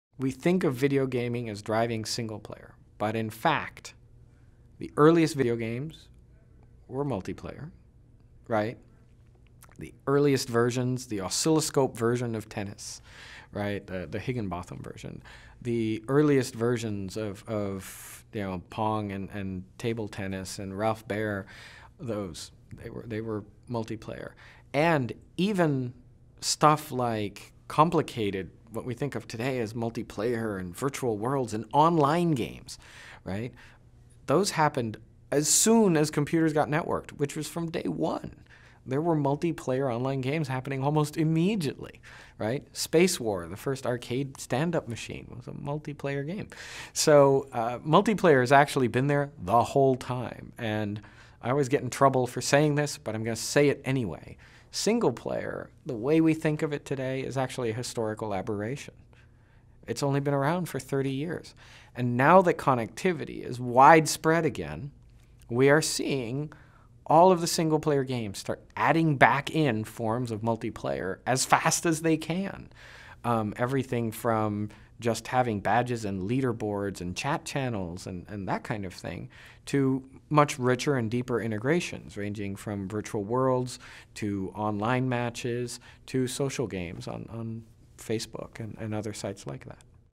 Raph Koster - Early Games Mulitplayer
Videogames have consistently pushed multiplayer gameplay ever since the early games. Single player games have drifted from what they used to be in that now they add on the multiplayer aspect to give connectivity to these games.

"We think of video gaming as driving single player but in fact the earliest video games were multiplayer."